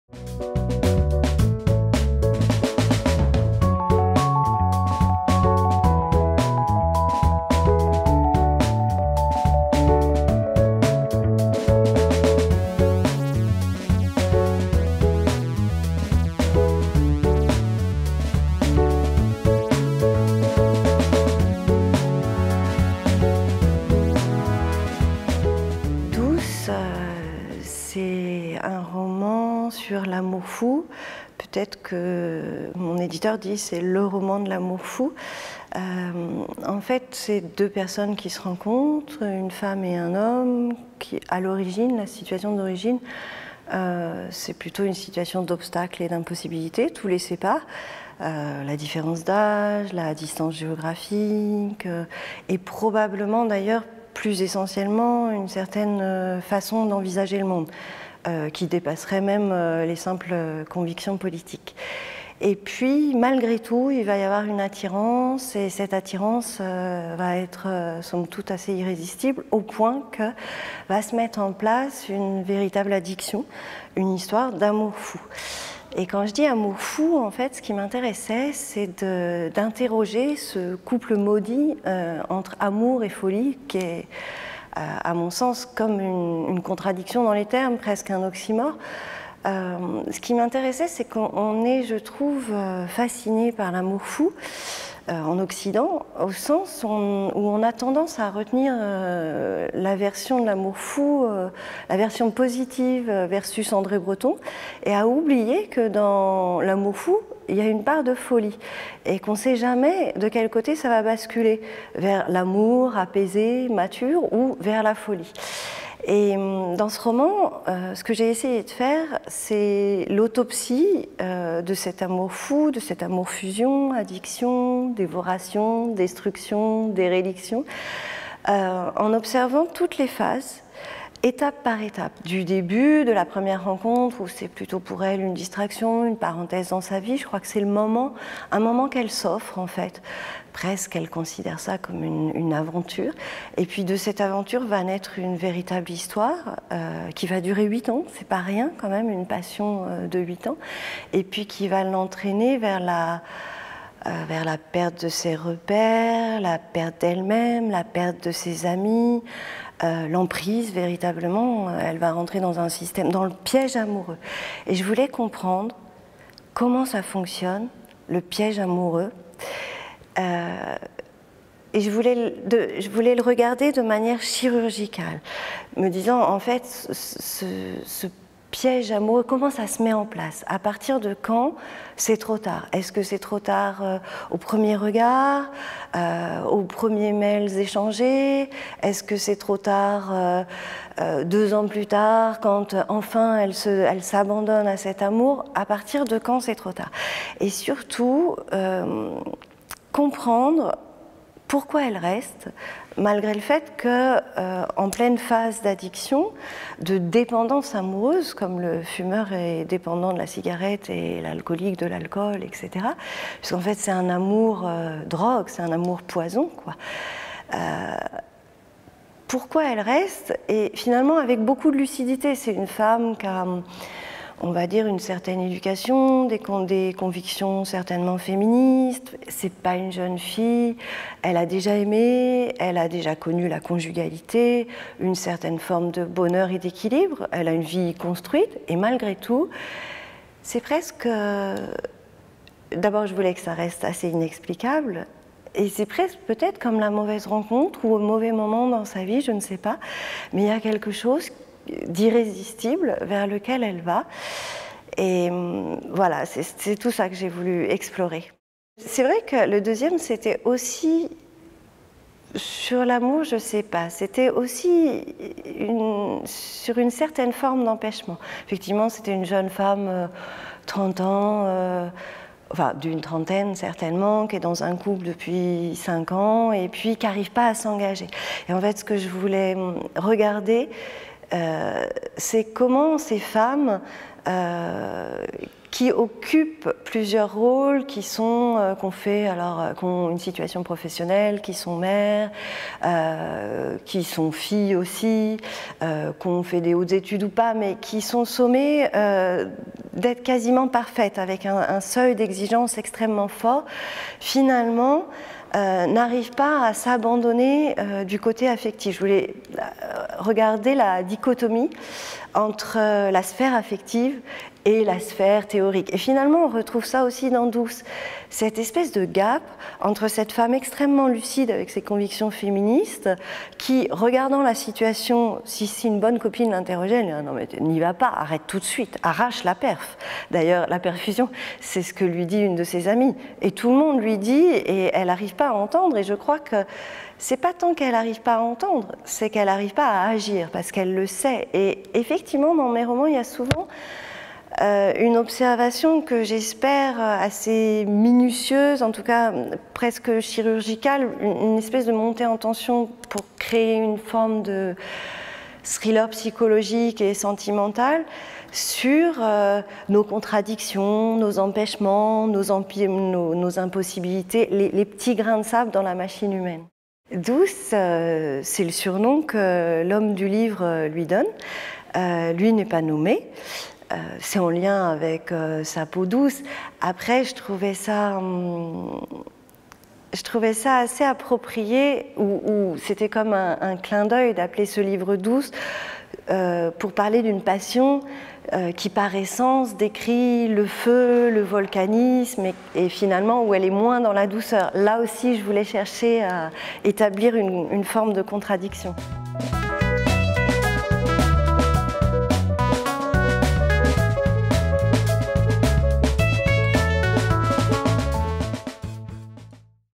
Sylvia Rozelier - Douce
À l'occasion du Livre sur la Place à Nancy, Sylvia Rozelier  vous présente son ouvrage "Douce" aux éditions  le Passage .